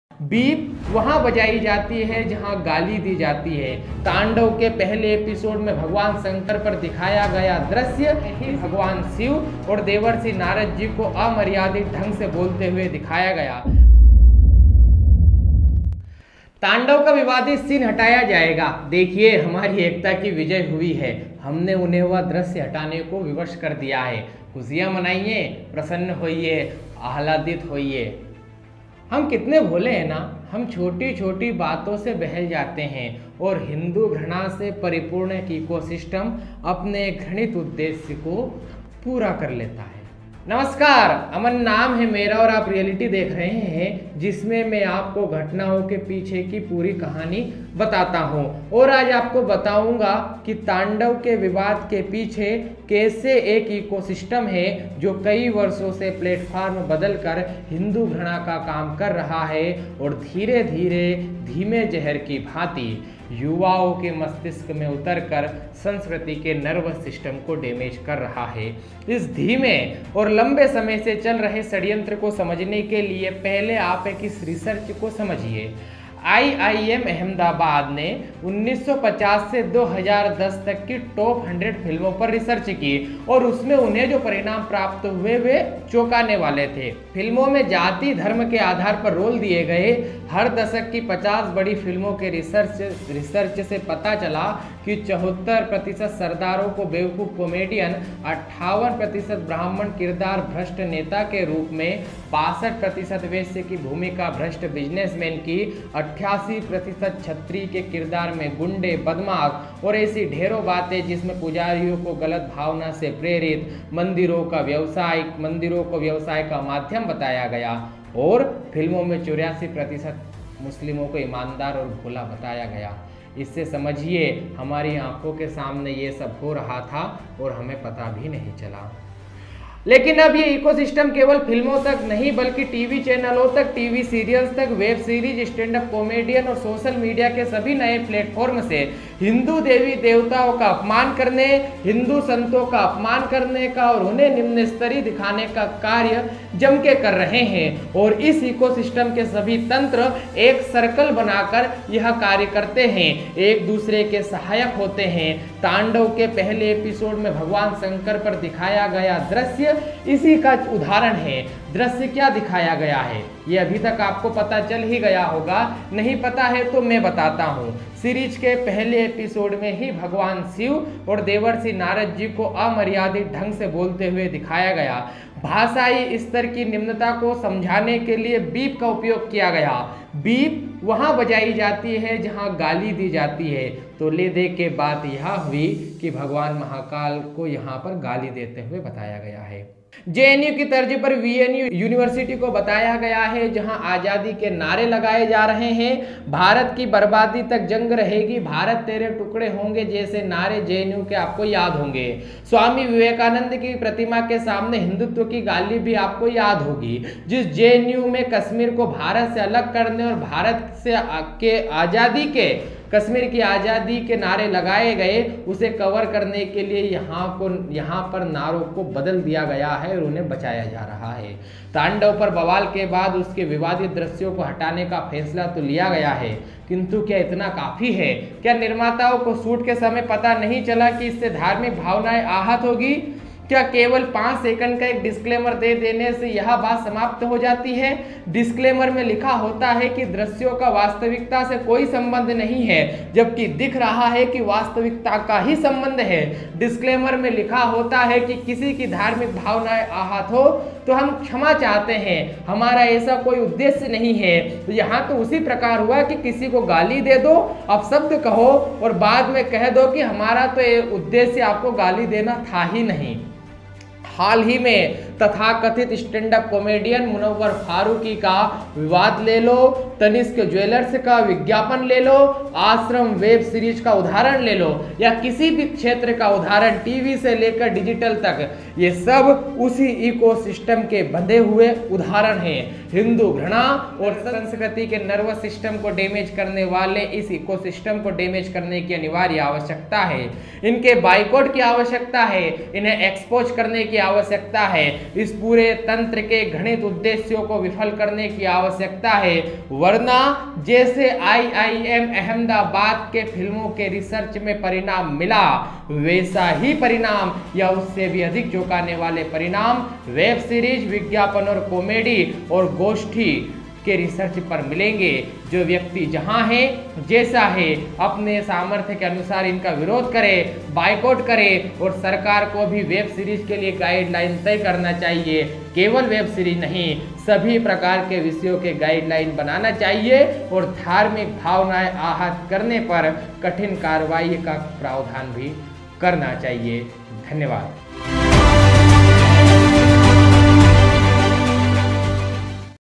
तांडव वेब सिरीज़ अकेली नहीं हैं पूरा बॉलीवुड हिन्दुफोबिक हैं ये वीडियो देखिये
तांडव में हिन्दू देवी - देवताओं को असभ्य बातें करते दिखाना, यह बॉलीवुड की कोई पहली करतूत नहीं हैं, जबकि हिन्दुफोबिक इकोसिस्टम इस कार्य को सदियो से कर रहा हैं।
देखिये ये वीडियो